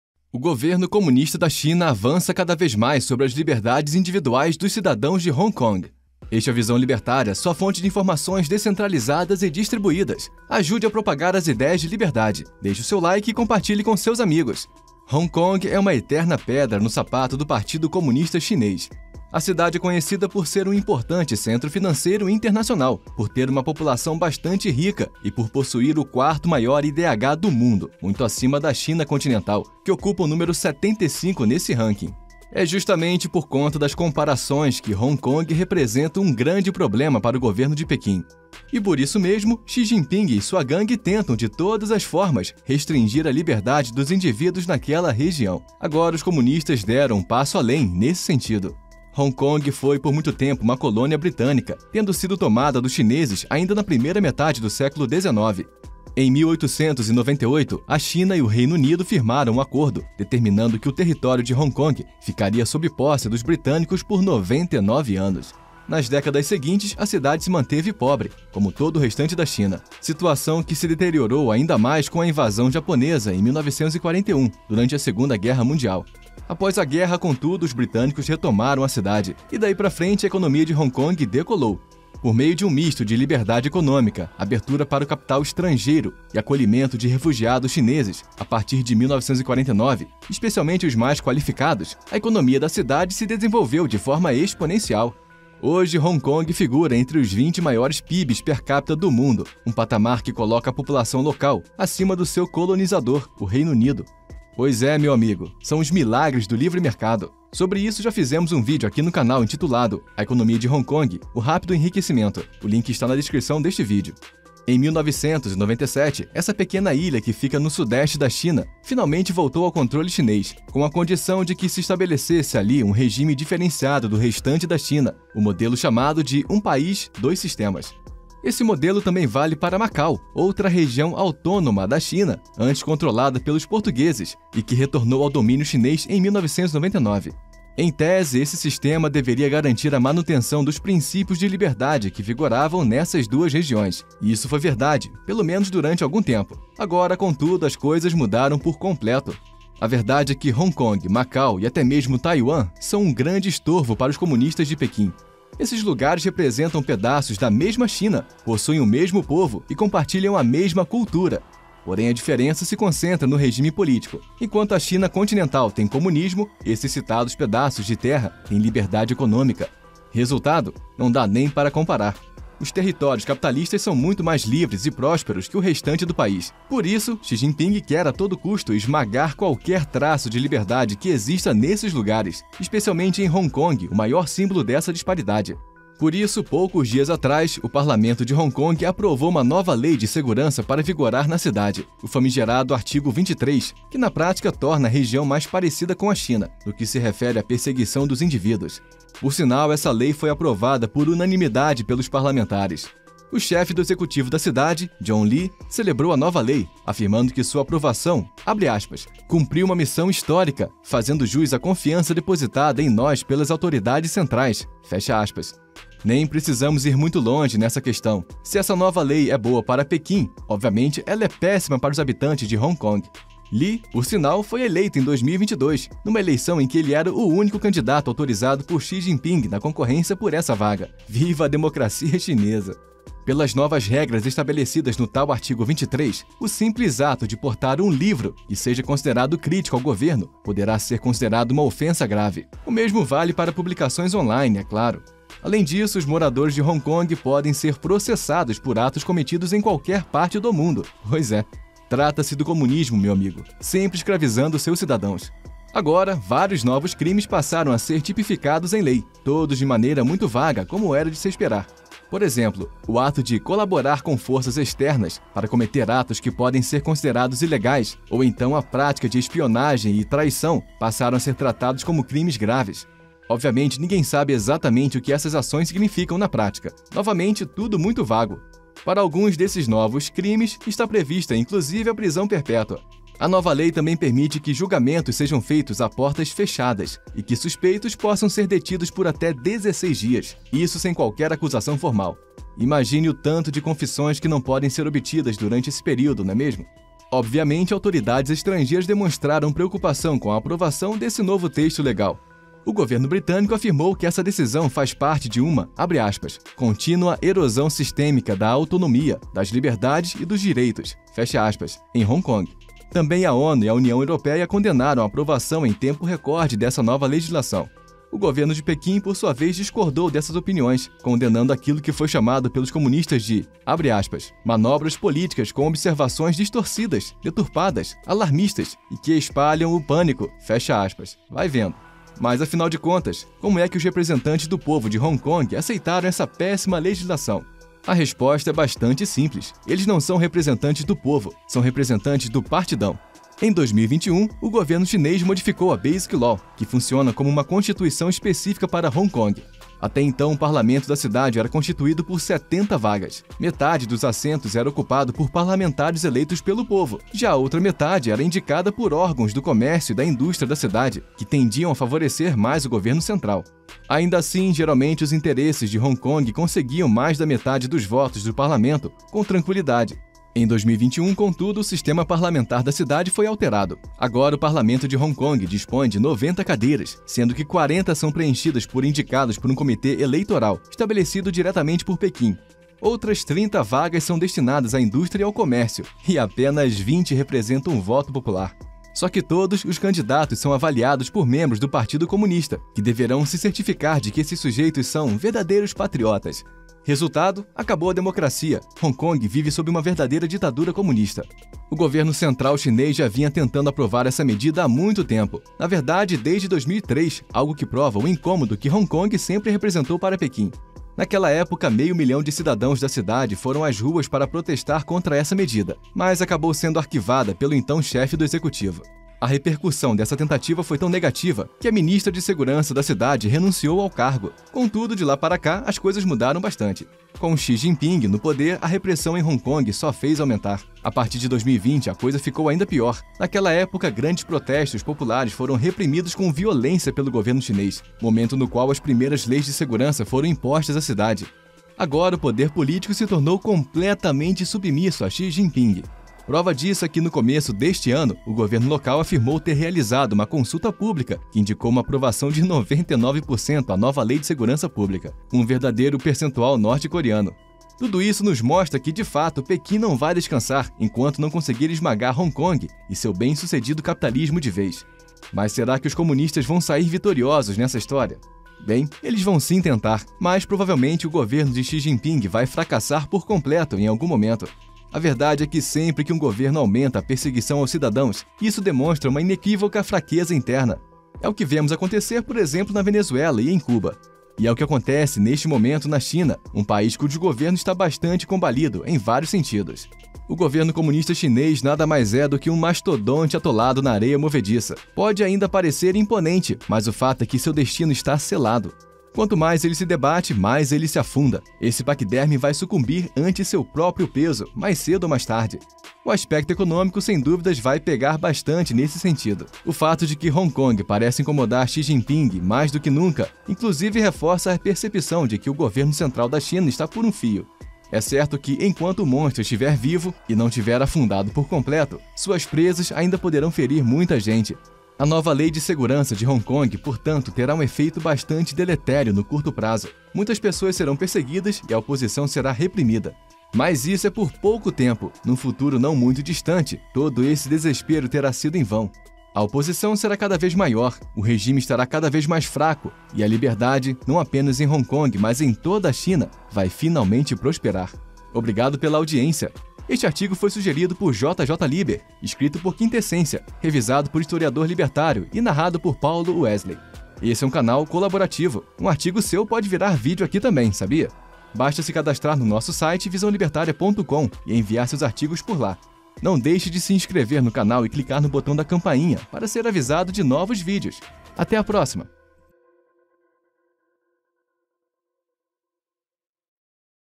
Governo da CHINA impõe LEI DE SEGURANÇA a HONG KONG com pena de PRISÃO PERPÉTUA para opositores
Ajude o canal:

16vmNcrA4Mvf7CaRLirAmpnjz1ZH3bWNkQ (Bitcoin)